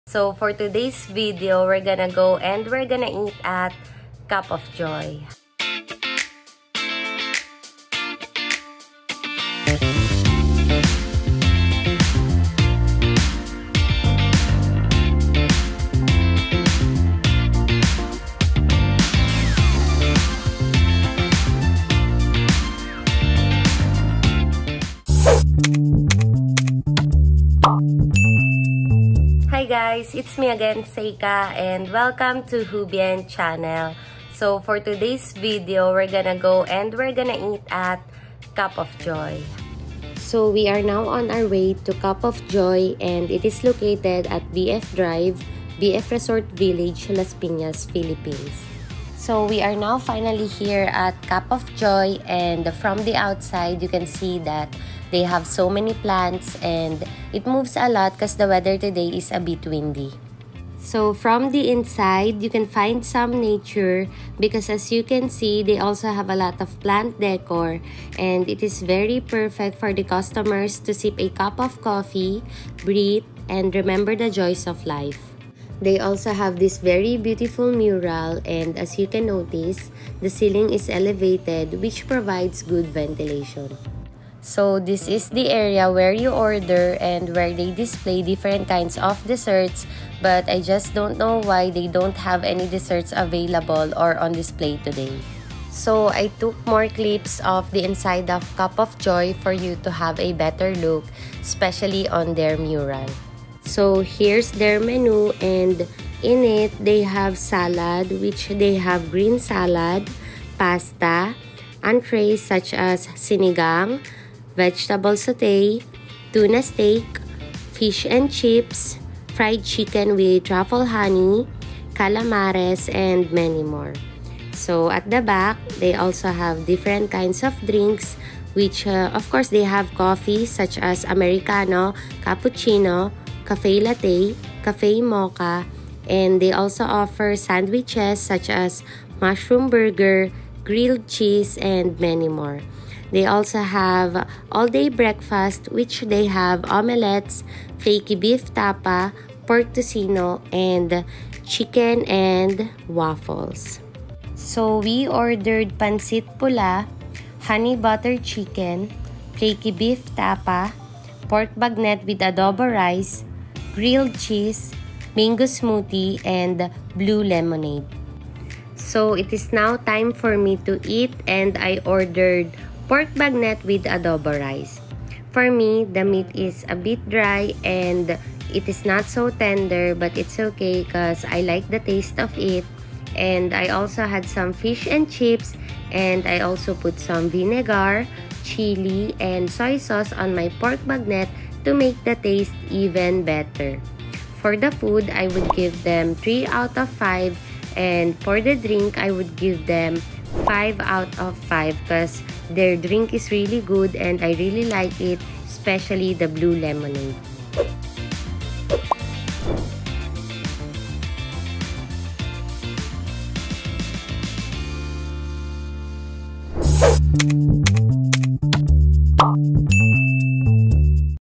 Trying out Cup of Joy with Seika| Las Pinas City, Manila, PH
This video follows Seika as she tries out Cup of Joy, a cafe in Las Pinas City, Manila, Philippines. The video showcases the restaurant unique atmosphere and menu, as well as Seika's experience trying out the food and drinks. It also provides viewers with a glimpse of the cafe. Join Seika as she embarks on a journey of discovery with Cup of Joy, a unique and exciting Menu. Experience the unique flavor combinations and explore the possibilities.

Follow HUBient and join us for more exciting things to come!

If you want to support out talents and our work, pls click on link below to donate.